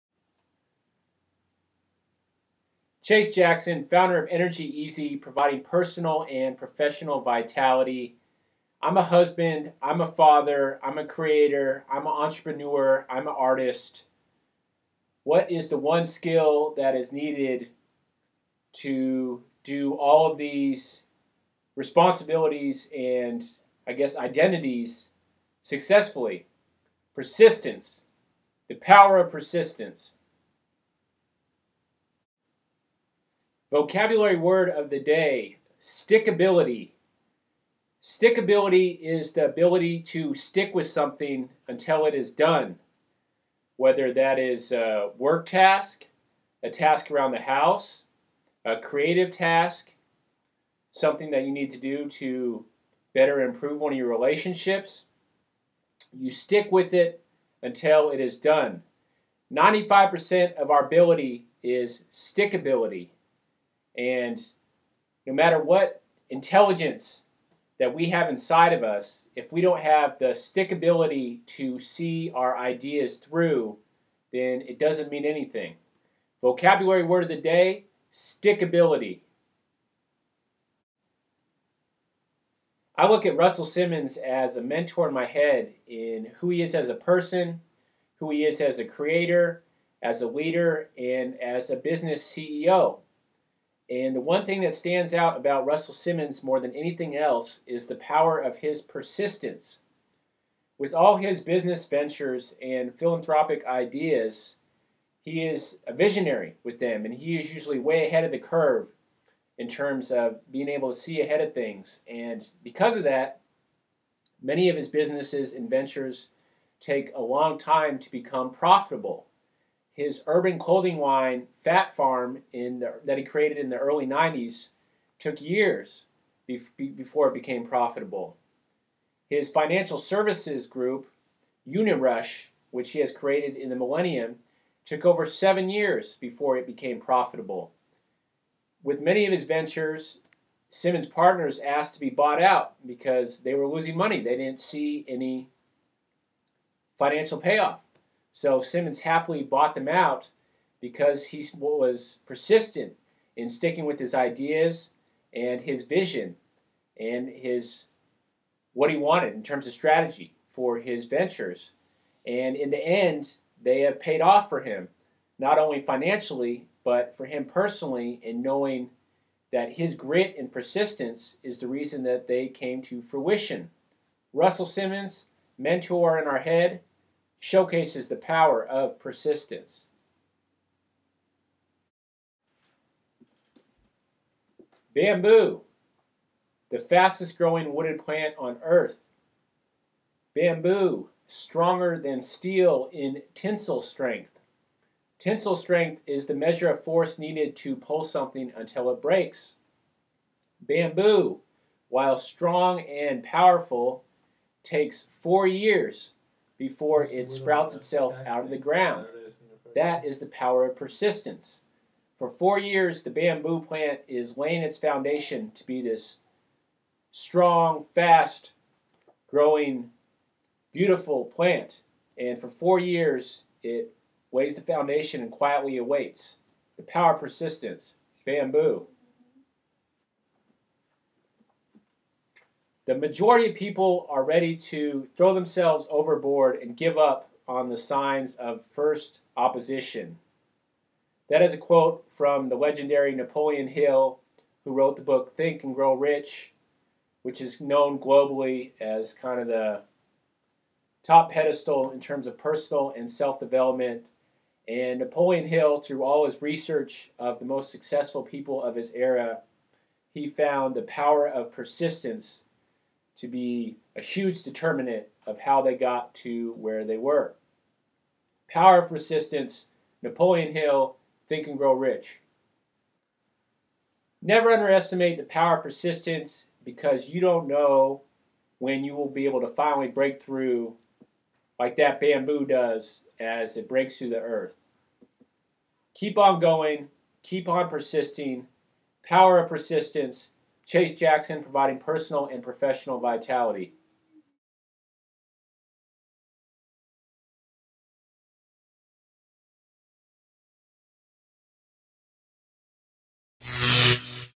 Russell Simmons- Power of Persistence- Motivation
True greatness can only be found through persistence until the end.  Providing personal and professional vitaliity